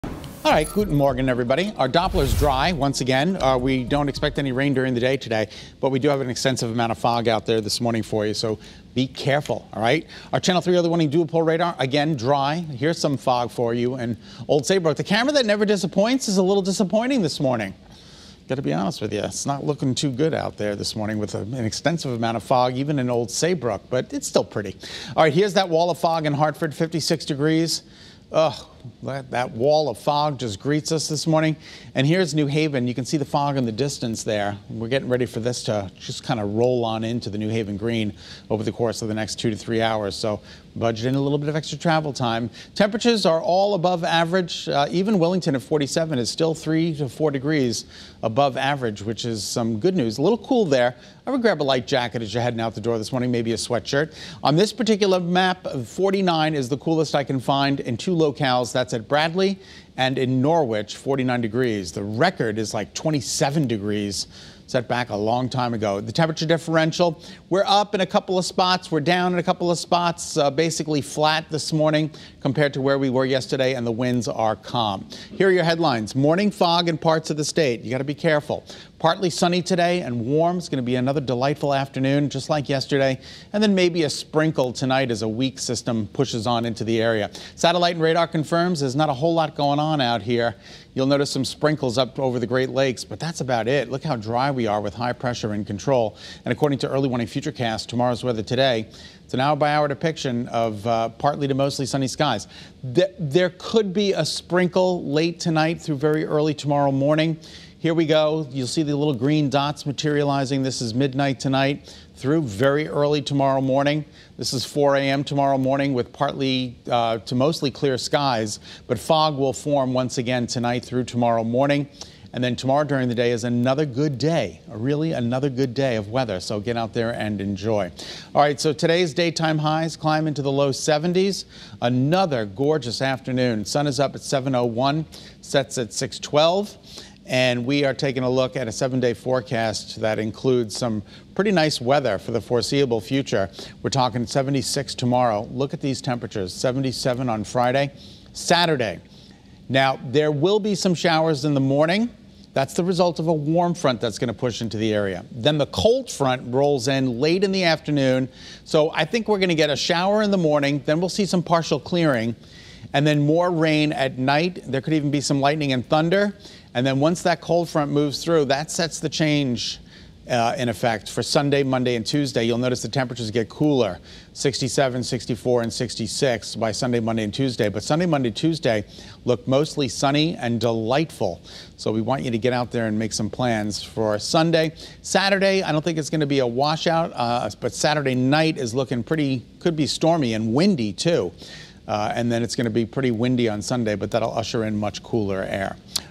FORECAST: Extensive amount of fog out there
Meteorologist Scot Haney reported an extensive amount of fog on Wednesday morning. The rest of the day and week, however, looks unseasonably warm. Here is his Wednesday morning forecast.